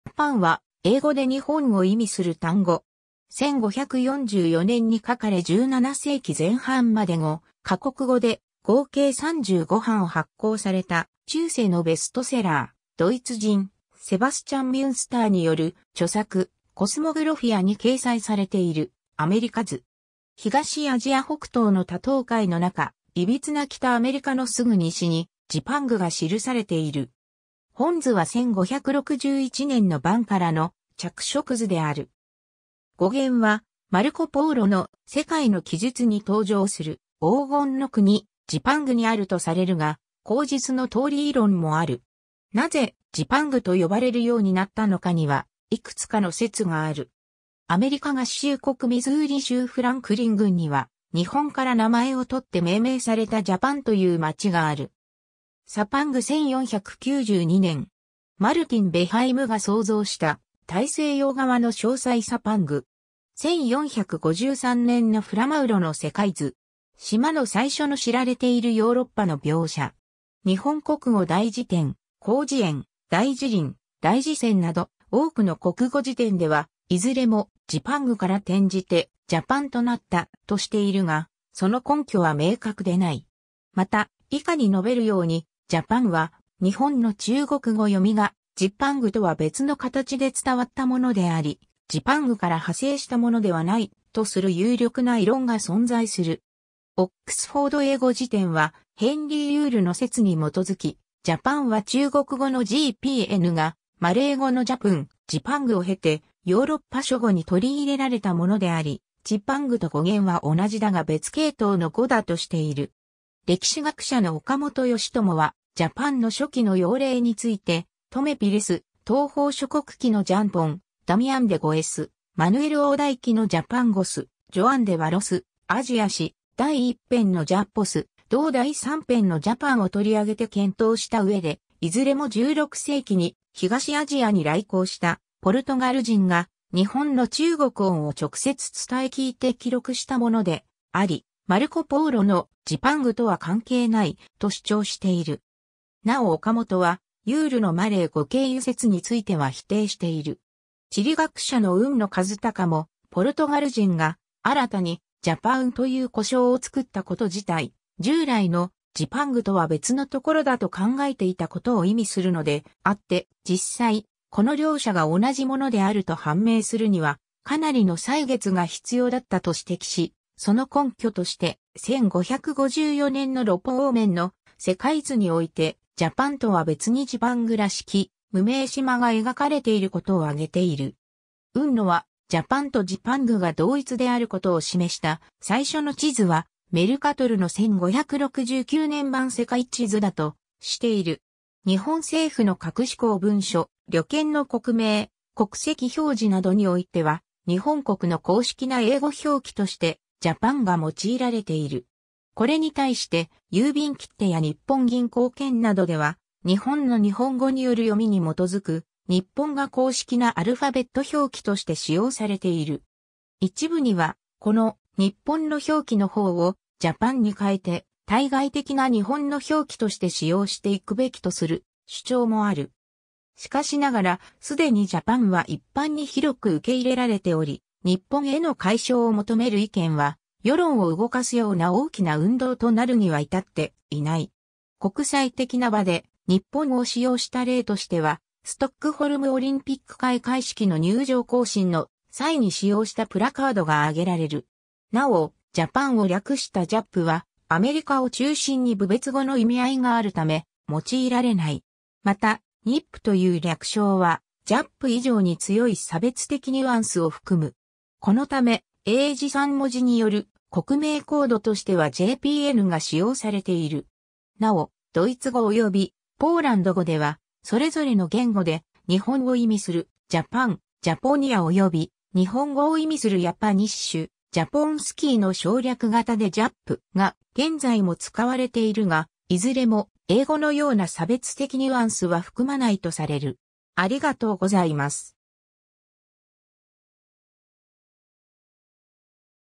ジャパン
ジャパン（英語: Japan）は、英語で日本を意味する単語。
 1544年に書かれ17世紀前半まで5か国語で合計35版発行された中世のベストセラー、ドイツ人セバスチャン・ミュンスターによる著作「Cosmographia」に掲載されているアメリカ図。
東アジア北東の多島海の中、歪な北アメリカのすぐ西にジパングが記されている。
本図は1561年の版からの着色図である。
 語源はマルコ・ポーロの「世界の記述」（東方見聞録）に登場する「黄金の国・ジパング (ZIPAN・ZIPANGU) 」にあるとされるが、後述の通り異論もある。
なぜ「ジパング」と呼ばれるようになったのかには、いくつかの説がある。
 アメリカ合衆国ミズーリ州フランクリン郡には、日本から名前を取って命名されたジャパン（Japan）という町（非法人地域）がある。
 Cipangu（チパング）1492年。
マルティン・ベハイムが想像した大西洋側の詳細 Cipangu（チパング）。
1453年のフラ・マウロの世界図。
島の最初の知られているヨーロッパの描写。
 『日本国語大辞典』『広辞苑』『大辞林』『大辞泉』など、多くの国語辞典では、いずれも「ジパング」から転じて「ジャパン」となった、としているが、その根拠は明確でない。
また、以下に述べるように、「ジャパン」は「日本」の中国語読みが「ジパング」とは別の形で伝わったものであり、「ジパング」から派生したものではない、とする有力な異論が存在する。
 『オックスフォード英語辞典』は、ヘンリー・ユールの説に基づき、「ジャパン」は中国語の Jih-pŭn がマレー語の Jăpung, Japang を経てヨーロッパ諸語に取り入れられたものであり、 Chipangu （チパング）と語源は同じだが別系統の語だとしている。
 歴史学者の岡本良知は、「ジャパン」の初期の用例について、トメ・ピレス『東方諸国記』（1514年筆）の Jampon 、ダミアン・デ・ゴエス『マヌエル王代記』（1562年筆・1567年刊）の Japongos 、ジョアン・デ・バロス『アジア史』第1編（1552年刊）の Japões 、同第3編（1556年刊）の Japam を取り上げて検討した上で、いずれも16世紀に東アジアに来航したポルトガル人が、「日本」の中国音（広東音）を直接伝え聞いて記録したものであり、マルコ・ポーロの「ジパング」とは関係ない、と主張している。
なお岡本は、ユールのマレー語経由説については否定している。
 地理学者の海野一隆も、「ポルトガル人が新たに「ジャパウン」という呼称を作ったこと自体、従来の「ジパング」とは別のところだと考えていたことを意味するのであって、実際、この両者が同じものであると判明するには、かなりの歳月が必要だった」と指摘し、その根拠として、1554年のロポ・オーメンの世界図において、ジャパン（大陸と陸続きになっている）とは別にジパングらしき無名島が描かれていることを挙げている。
海野は、ジャパンとジパングが同一であることを示した最初の地図は、メルカトルの1569年版世界地図（en:Mercator 1569 world map）だとしている。
 日本政府の各種公文書、旅券の国名・国籍表示などにおいては、日本国の公式な英語表記として「JAPAN」が用いられている。
これに対して、郵便切手や日本銀行券などでは「日本」の日本語による読み（発音）に基づく「NIPPON」が公式なアルファベット表記として使用されている。
 一部にはこの「NIPPON」の表記の方を「JAPAN」に代えて対外的な日本の表記として使用していくべきとする主張もある。
しかしながら既にJAPANは一般に広く受け入れられており、「NIPPON」への改称を求める意見は、世論を動かすような大きな運動となるには至っていない。
国際的な場で「NIPPON」を使用した例としては、ストックホルムオリンピック開会式の入場行進の際に使用したプラカードが挙げられる。
 なおJAPANを略したJAP（ジャップ）は、アメリカを中心に侮蔑語の意味合いがあるため、用いられない。
また NIP（ニップ）という略称は、JAP以上に強い差別的ニュアンスを含む。
このため英字3文字による国名コードとしてはJPNが使用されている。
なお、ドイツ語およびポーランド語ではそれぞれの言語で「日本」を意味するJapan（ヤーパン）/Japonia（ヤポニア）および「日本語」を意味するJapanisch（ヤパーニッシュ）/Japoń...